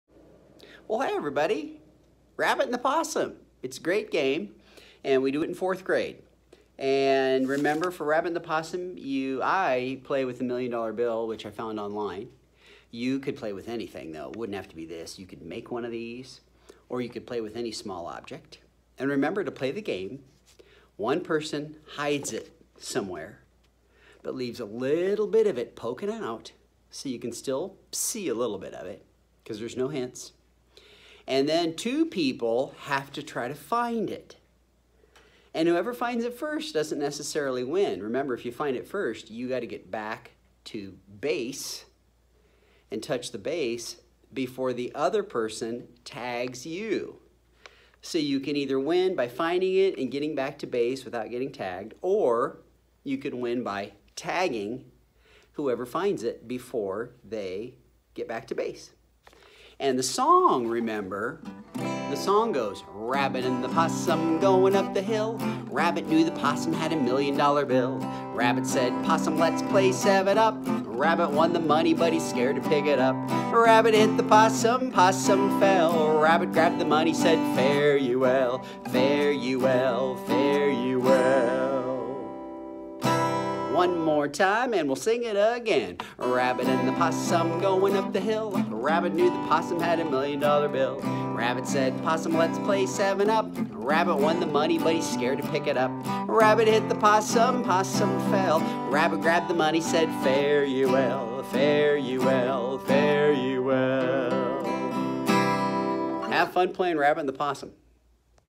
10. Rabbit and the Possum
For the 4th and 5th Graders. A "find It" game. Could you find the MILLION DOLLAR BILL?? (Don't worry. It's fake.)